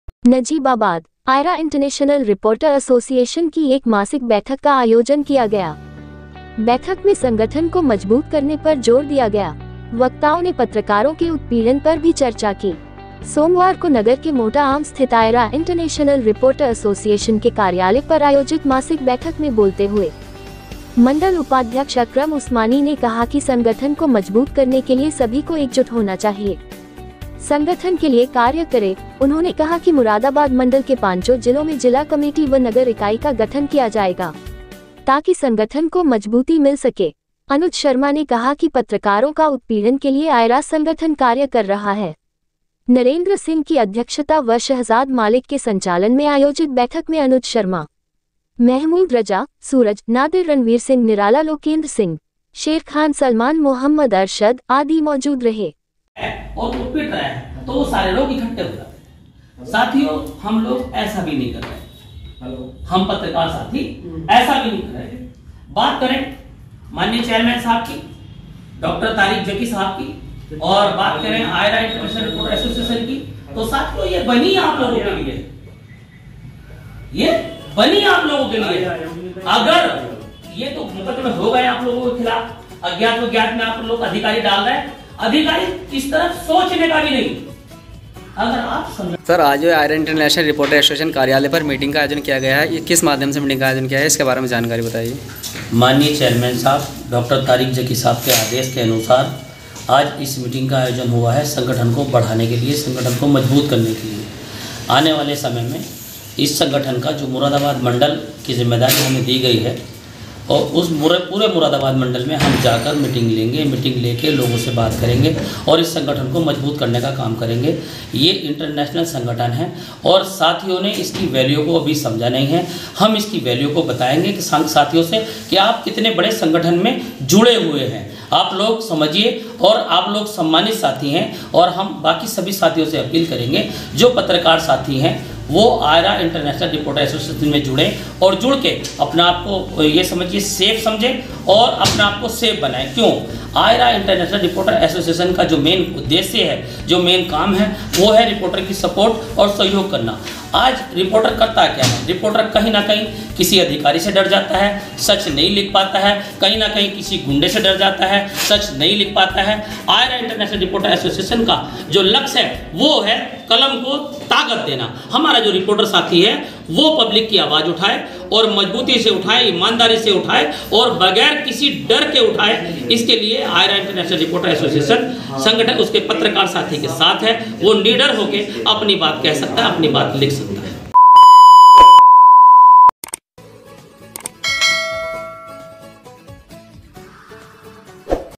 नजीबाबाद आईरा इंटरनेशनल रिपोर्टर एसोसिएशन की एक मासिक बैठक का आयोजन किया गया बिजनौर मोहम्मद अरशद
*नजीबाबाद। आईरा इंटरनेशनल रिपोर्टर एसोसिएशन की एक मासिक बैठक का आयोजन किया गया*

बिजनौर से मोहम्मद अरशद की रिपोर्ट

 बैठक में संगठन को मजबूत करने पर जोर दिया गया। वक्ताओं ने पत्रकारों के उत्पीड़न पर भी चर्चा की।
     सोमवार को नगर के मोटा आम स्थित आईरा इंटरनेशनल रिपोर्टर एसोसिएशन के कार्यालय पर आयोजित मासिक बैठक में बोलते हुए मंडल उपाध्यक्ष अकरम उस्मानी ने कहा कि संगठन को मजबूत करने के लिए सभी को एकजुट होना चाहिए, संगठन के लिए कार्य करे, उन्होंने कहा कि मुरादाबाद मंडल के पांचो जिलों में जिला  कमेटी व नगर इकाई का गठन किया जाएगा, ताकि संगठन को मजबूती मिल सके। अनुज शर्मा ने कहा कि पत्रकारों का उत्पीड़न के लिए आईरा संगठन कार्य कर रहा है। नरेंद्र सिंह की अध्यक्षता व शहजाद मालिक के संचालन में आयोजित बैठक में अनुज शर्मा, महमूद रजा, सूरज, नादिर रणवीर सिंह निराला लोकेंद्र सिंह शेरखान सलमान मोहम्मद अरशद,आदि मौजूद रहे।